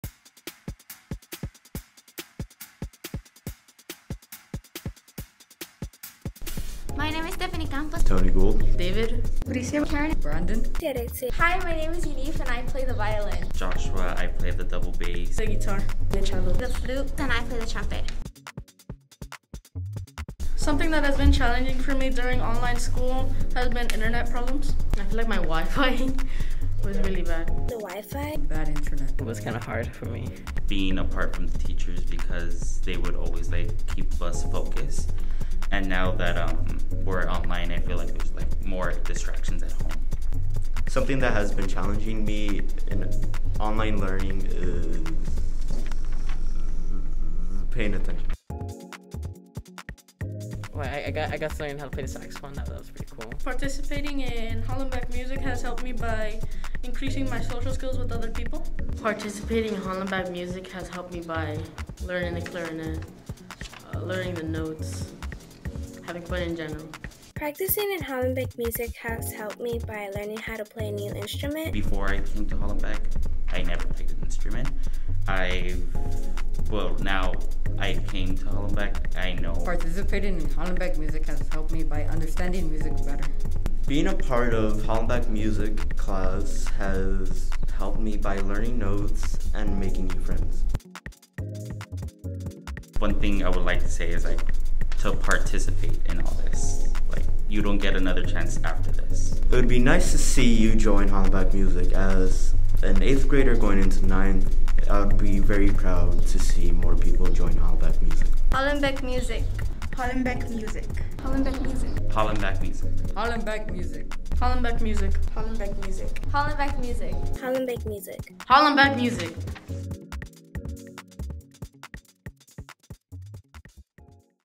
HMS 2021 Spring Music Interviews
Students from the Class of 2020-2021 Band/Orchestra/Mariachi share their thoughts on this Virtual/In-Person school year.